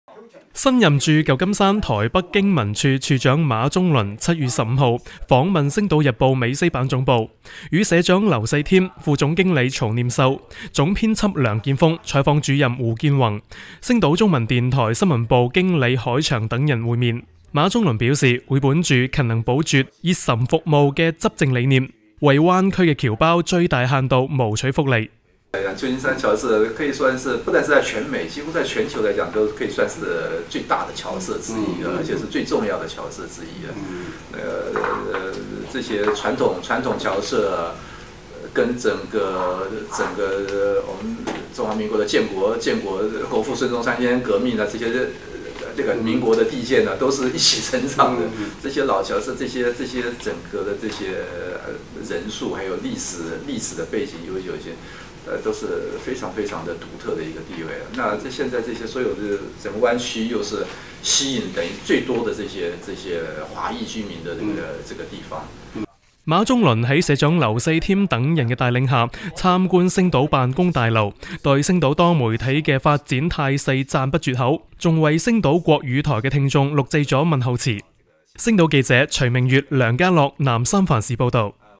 (粵)新任台北經文處處長訪星島
7月8日剛剛走馬上任的駐舊金山台北經文處處長馬鍾麟7月15號訪問星島日報美西版總部，與社長劉世添、副總經理曹念秀、總編輯梁建鋒、採訪主任胡建宏、星島中文電台新聞部經理海翔等人會面，除分享自己的從政之路外，馬鍾麟亦表態希望本著「勤能補拙、熱忱服務」的執政理念，為灣區的僑胞最大限度謀取福利。舊金山僑社不但在全美，幾乎在全球來講都是都可以稱得上是最大以及最重要的僑社之一。傳統僑社與國父孫中山先生革命等事件一起成長壯大，老僑社整合了人數以及歷史背景，如今在灣區佔有獨特的地位。整個灣區又是吸引眾多華裔居民的地方。馬鍾麟在社長劉世添等人的帶領下參觀星島辦公大樓，對星島多媒體的發展態勢讚不絕口，還為星島國語電台的聽眾錄製了問候辭。 問候辭 星島記者徐明月、梁嘉樂南舊金山市報道